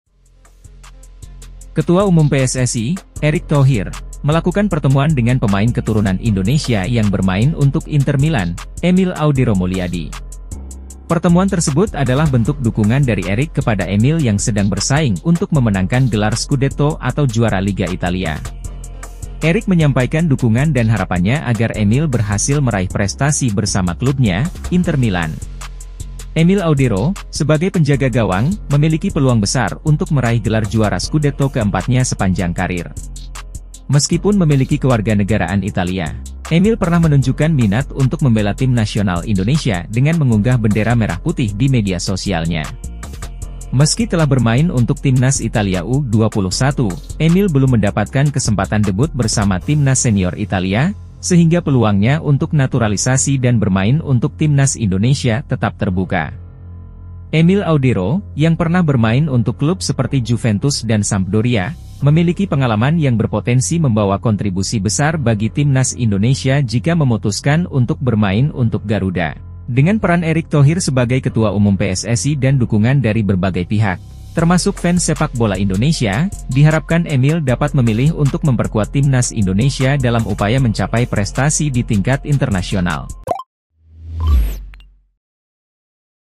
Erick Thohir bertemu pemain keturunan Indonesia di Inter Milan Emil Audero
Untuk berita lainnya kunjungi website kami di